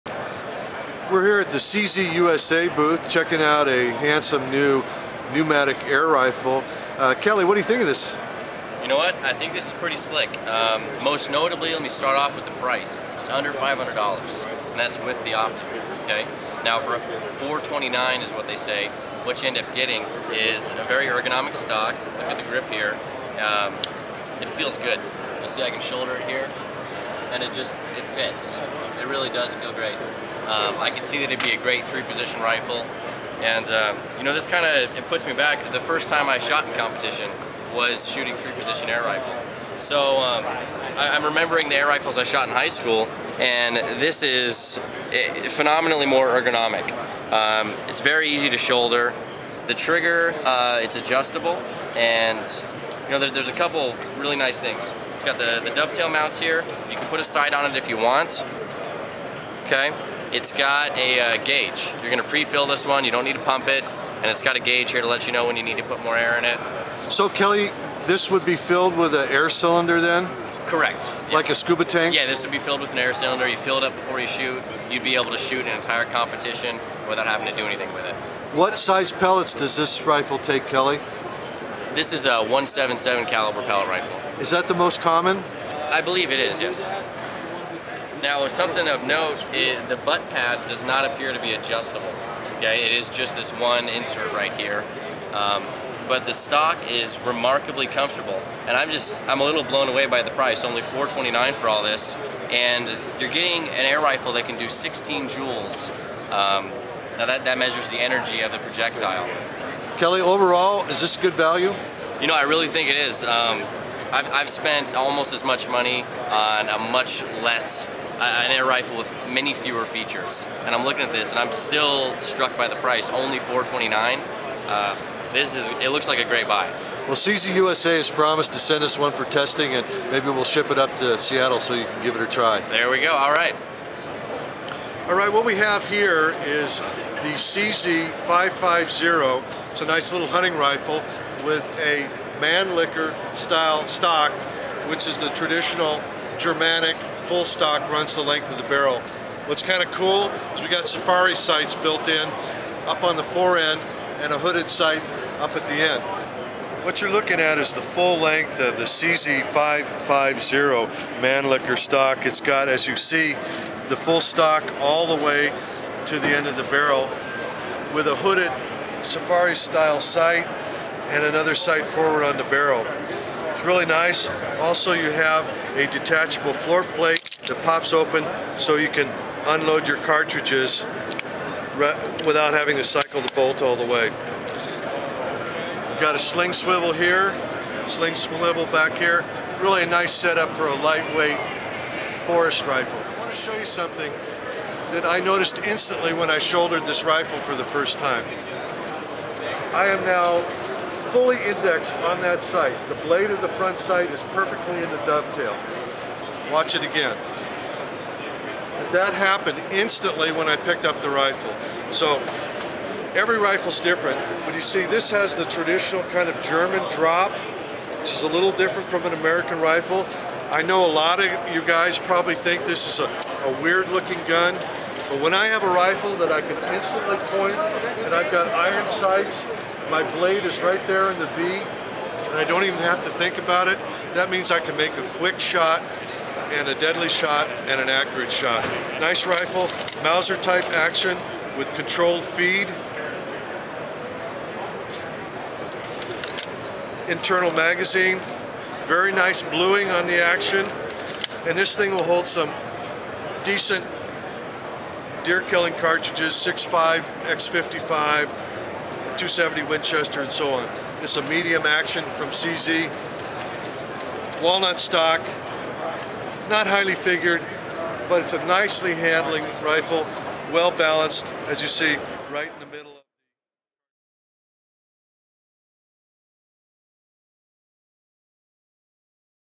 CZ for 2011 -- 200S Air Rifle and CZ 550 FS Mannlicher
We spotlight two great offerings from CZ-USA. First Kelly Bachand reviews the new-for-2011 CZ 200 S Air Rifle. Priced at just $429.00 including scope, this is a good value for a match-grade, 16-Joule .177 air rifle. Kelly found the gun very ergonomic, with nice features. Next, we show CZ's handsome 550 FS hunting rifle. With traditional Bavarian Mannlicher stock, this rifle points superbly -- the sights almost align themselves. Lightweight (7.2 lbs.) and easy to handle, this is a great example of a classic style field hunter. At $848.00, it compares favorably with custom rifles costing hundreds more.